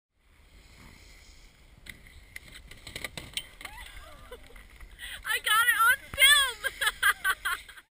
Skier Fails at Breck!
yup thats me righter.  kinda split this rail bad and i didnt like it.